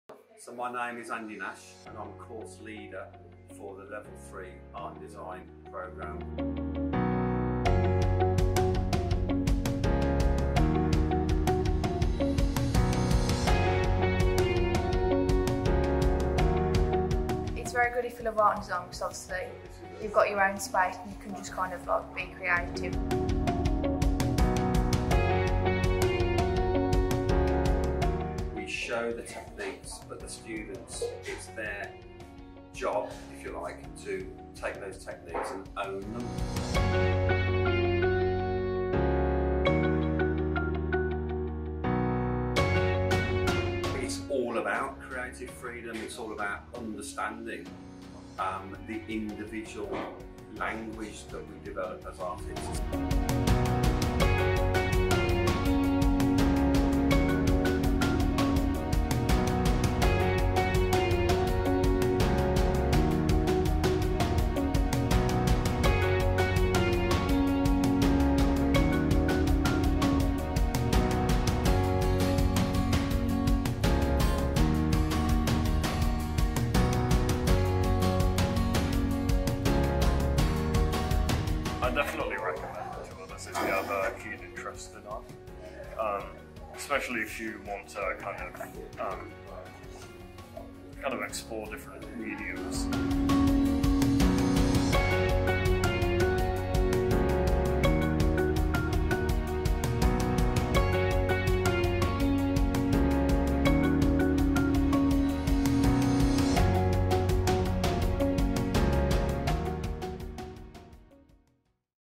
CreateYourCareer in Art & Design
Ben Wilson from our Level 3 TV, Film and Journalism course recently got in touch with us after making a fantastic promotional video for our Art & Design department at Lichfield College! 🎥

After choosing to focus on the Art department, Ben worked in a group with Tess Ward and Aidan Faulkner to capture footage of lecturer Andy Nash, some of our current students and our facilities to produce this great piece of work! 👏

If you are looking to CreateYourCareer in Art & Design, check out our courses and start with us this September 🎨